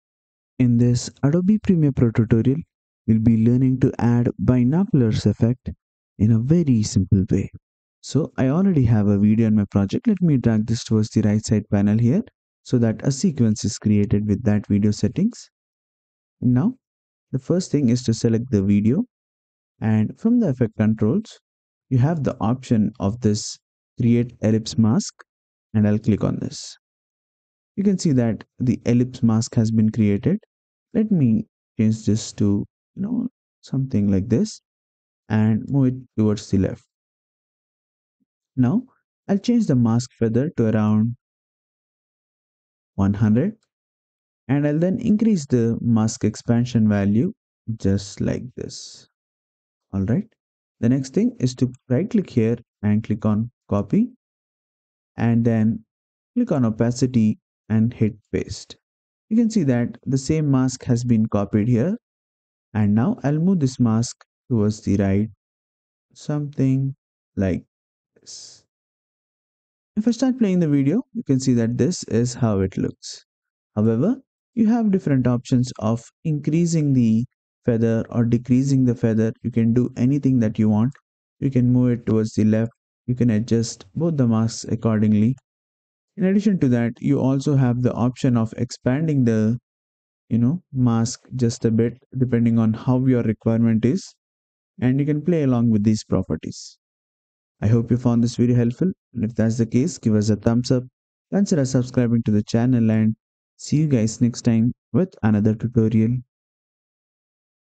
Binoculars Effect - Adobe Premiere Pro Tutorial
In this adobe premiere pro tutorial, we will be learning to add binoculars effect to our clip in a very simple and easy way. We will be learning to add binoculars effect by creating an ellipse mask on the footage and adjust the size and feather to it. Also, we will duplicate the same mask and move it to the side so that it looks like a binoculars.